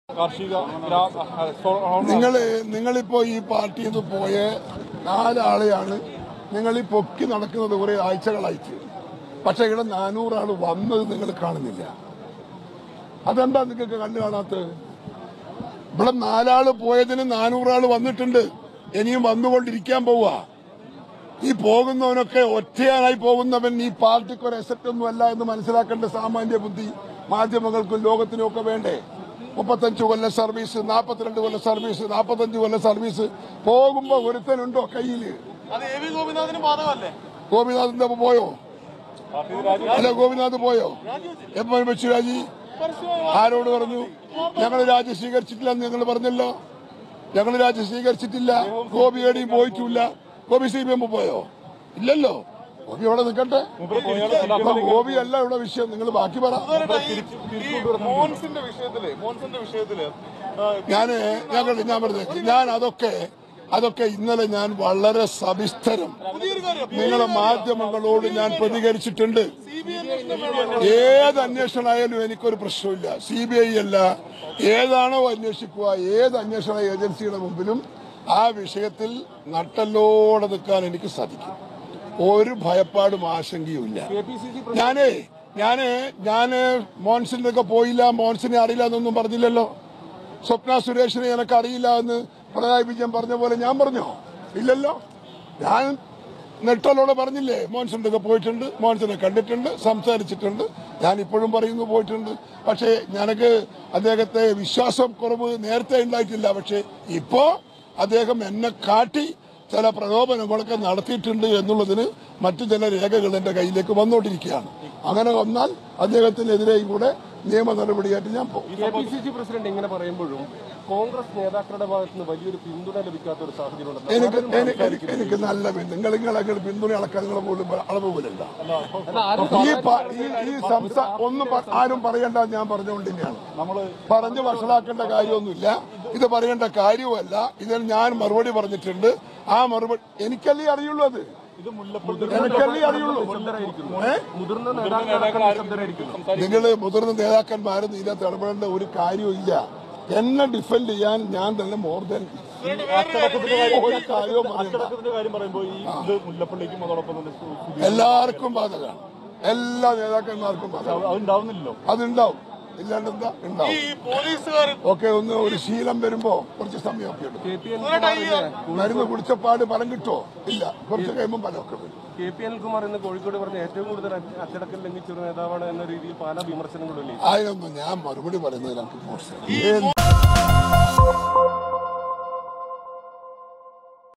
' പിണറായി വിജയൻ സ്വപ്ന സുരേഷിനെ അറിയില്ലെന്ന് പറഞ്ഞത് പോലെ മോൻസനെ അറിയില്ലെന്ന് ഞാൻ പറയില്ല '
പിണറായി വിജയൻ സ്വപ്ന സുരേഷിനെ അറിയില്ലെന്ന് പറഞ്ഞത് പോലെ മോൻസനെ അറിയില്ലെന്ന് ഞാൻ പറയില്ലെന്ന് കെ സുധാകരൻ